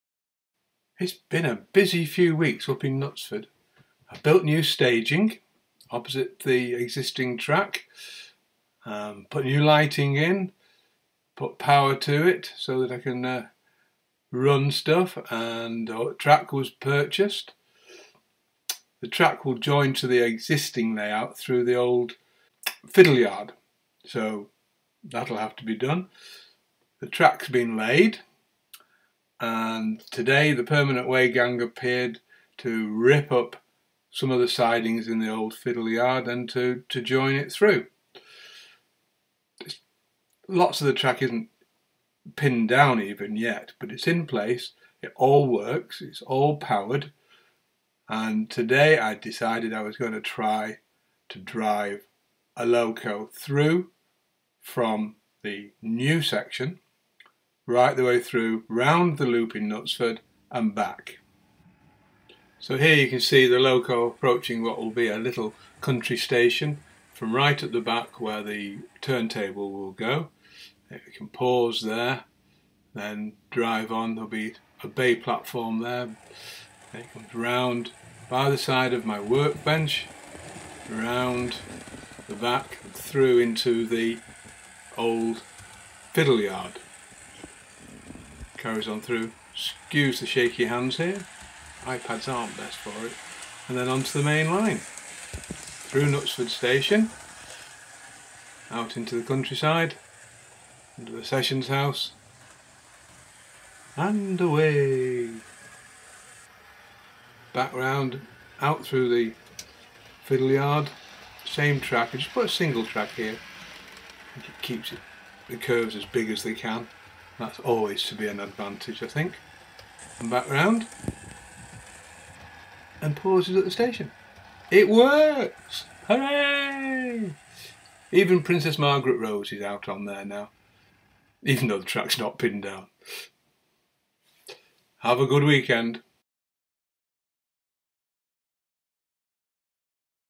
The First Run
I have been building a new extension to my existing OO scale layout. "Knutsford" This morning is the first time I have had it all connected up so I attempted to run a DMU (Diesel Multiple Unit) from the far reaches of the new section , and out onto the first phase of my layout - and back again!